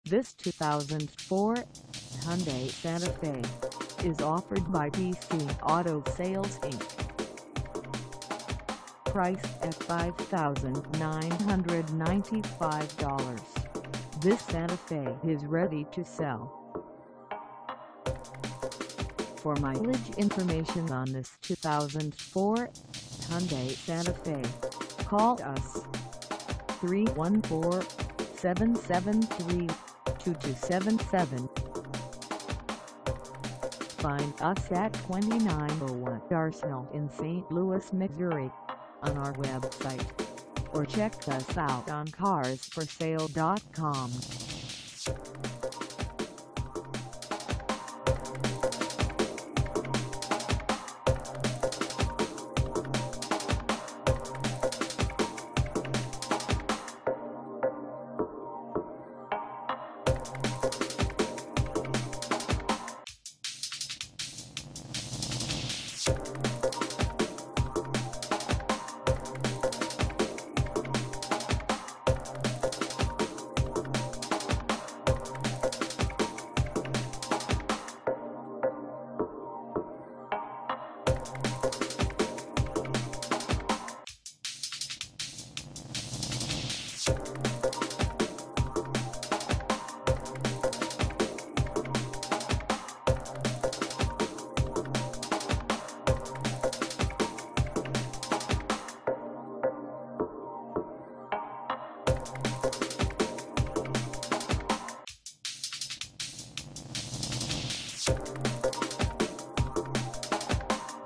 2004 Hyundai Santa Fe GLS 2.7L 4WD for sale in Saint Louis,
This 2004 Hyundai Santa Fe GLS 2.7L 4WD is for sale in Saint Louis, MO 63118 at DC Auto Sales Inc.
Engine: 6-Cylinder V6, 2.7L; DOHC
Transmission: Automatic
Color: Silver
Mileage: 0
Contact DC Auto Sales Inc 2901 Arsenal Saint Louis, MO 63118 to test drive this 2004 Hyundai Santa Fe GLS 2.7L 4WD today.